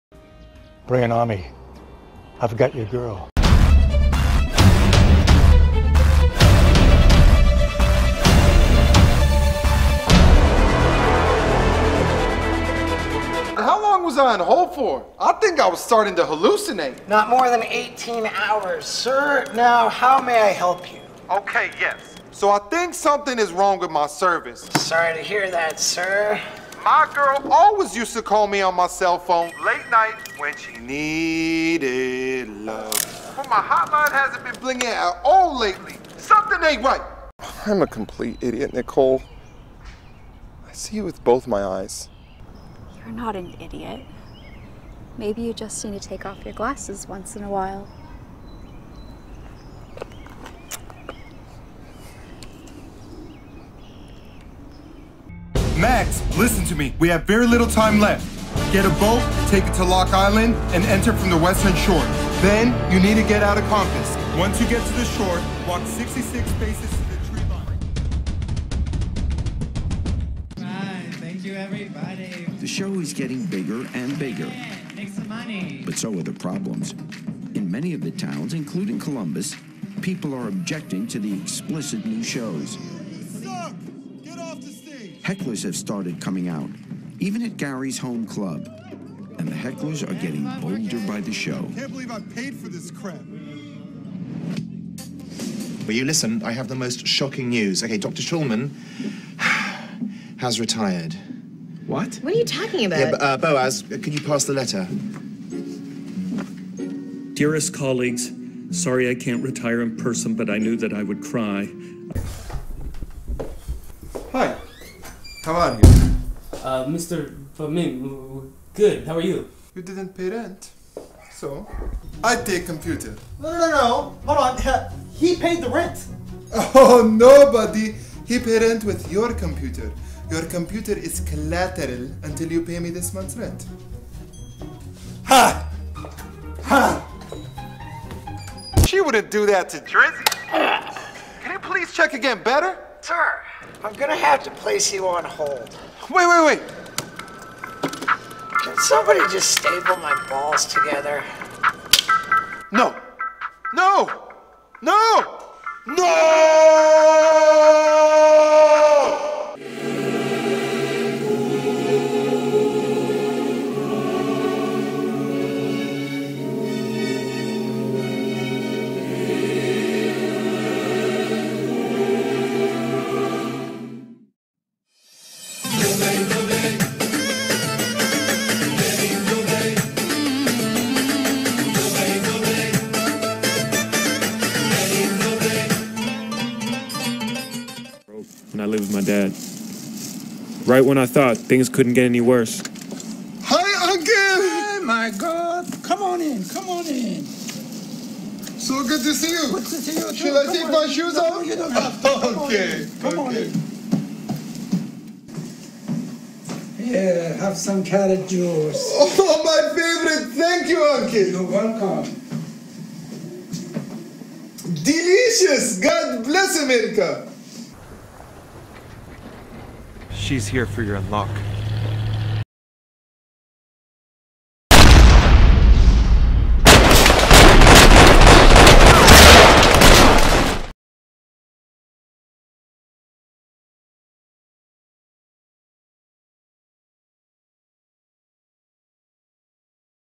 Acting Highlights from George Khouri Mr. GK
Mr. GK's acting career is limitless. As a method actor, he is able to play multiple characters seamlessly. He's respected for his broad range.

In this reel, you will see Mr. GK across from some of Hollywood's elite, including Mindy Kaling, and highly likable stars like Steve-O.